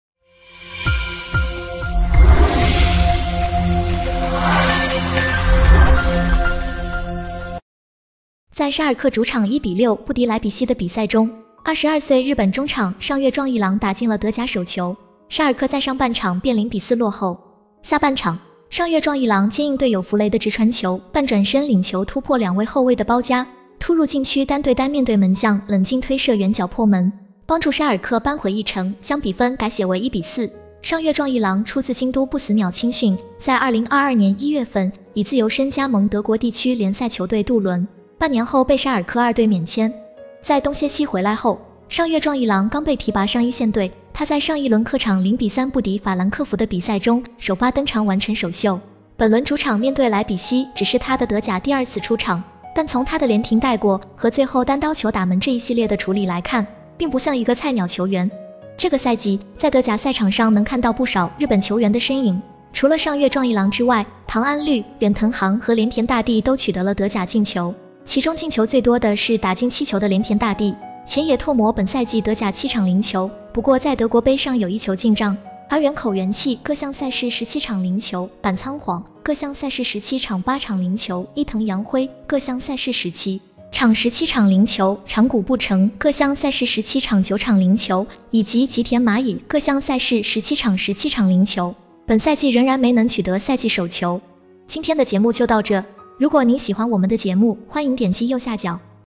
這位22歲的日本明星打破了1V2的目標，僅用兩場比賽就打進了德甲的第一球
在沙爾克主場1-6不敵萊比錫的比賽中，22歲日本中場上月壯一郎打進了德甲首球。沙爾克在上半場便0-4落后。下半場，上月壯一郎接應隊友弗雷的直傳球，半轉身領球突破兩位后衛的包夾，突入禁區單對單面對門將冷靜推射遠角破門，幫助沙爾克扳回一城，將比分改寫為1-4。